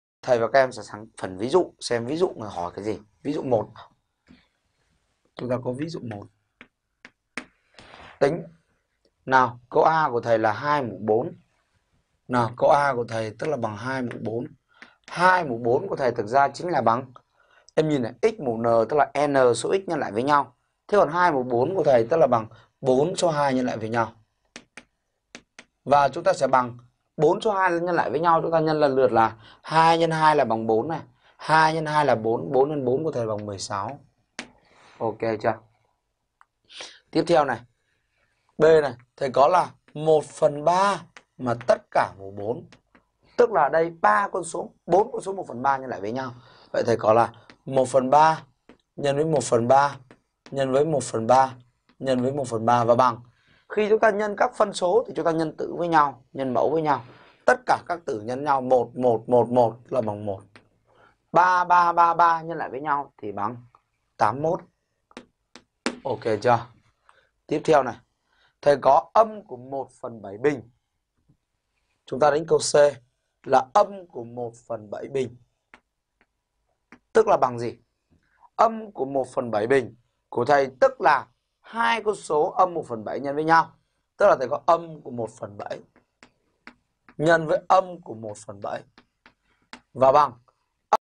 Lũy thừa của một số hữu tỉ - Số hữu tỉ - Số thực - Toán 7
Ví dụ 1. Tính
2^4; (1/3)^4; (−1/7)^2; (−0,2)^3; (−3/4)^0
Học online bài
LŨY THỪA CỦA MỘT SỐ HỮU TỈ